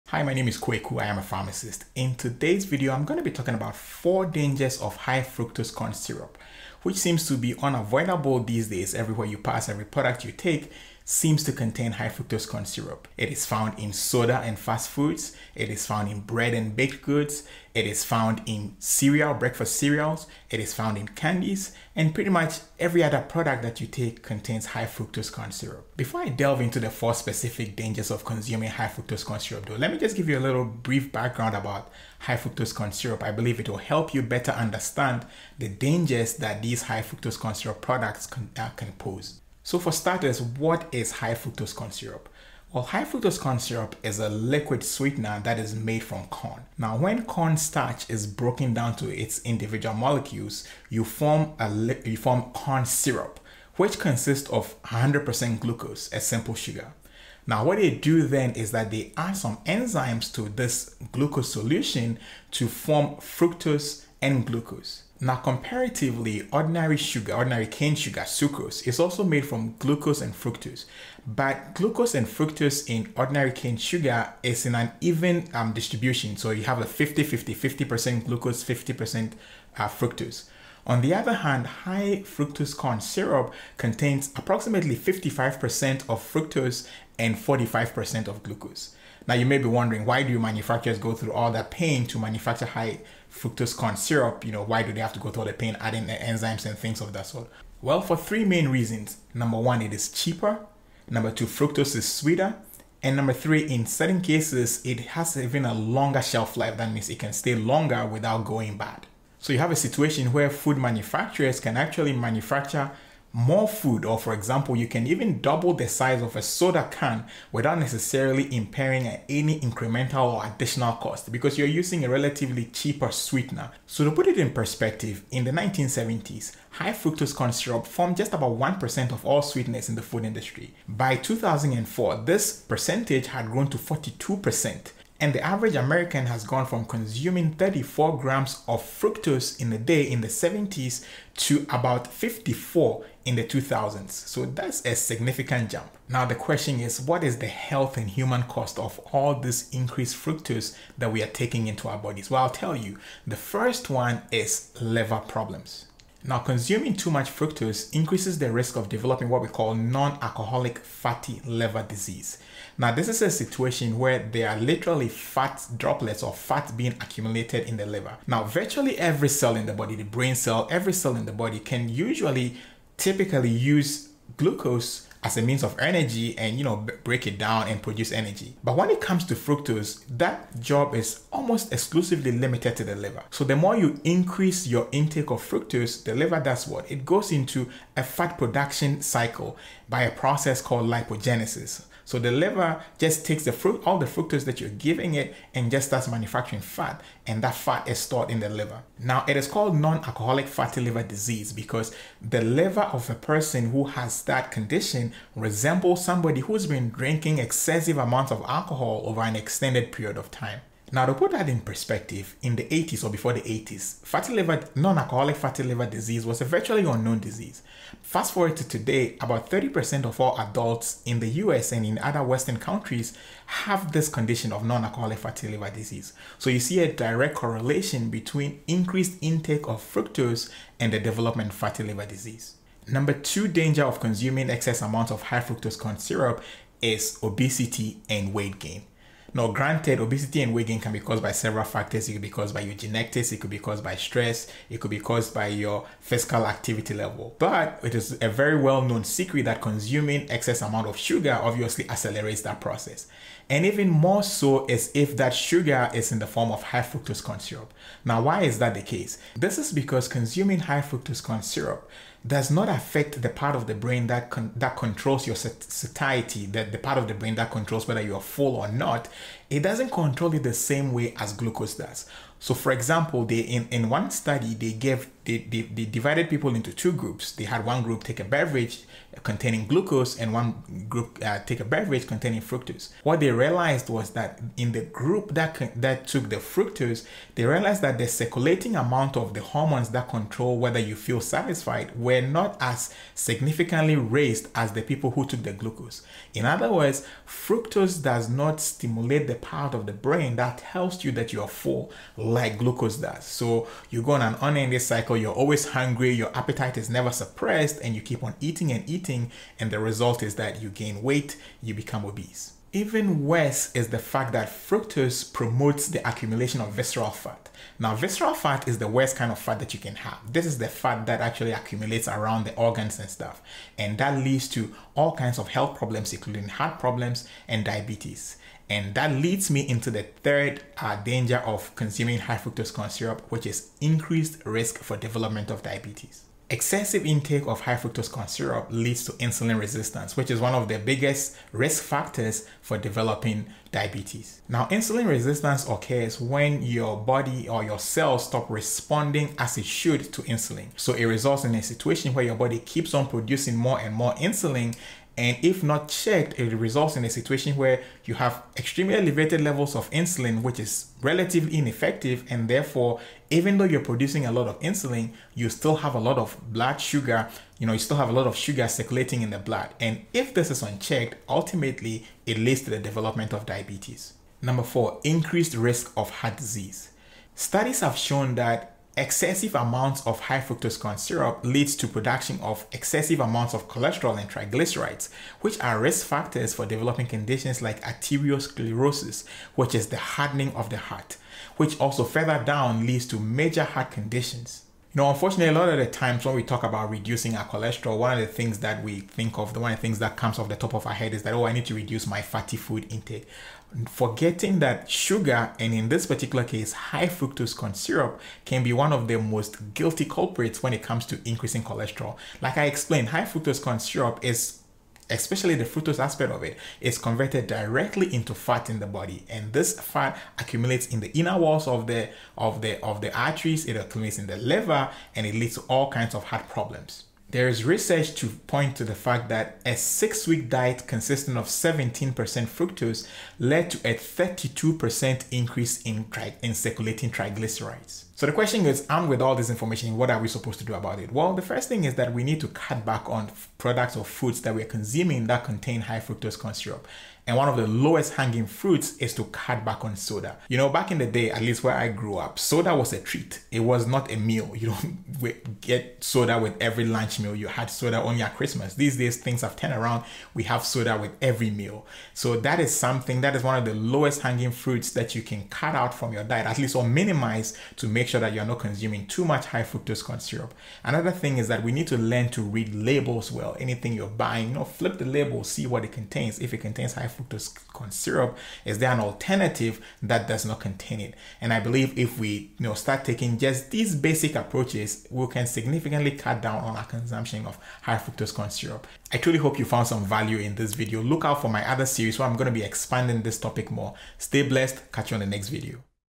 4 Dangers Of High Fructose Corn Syrup
High fructose corn syrup is a sweetener found in many processed foods. But what are the dangers of high fructose syrup? This video looks at four potential dangers of consuming high fructose corn syrup, including weight gain, diabetes, and heart disease.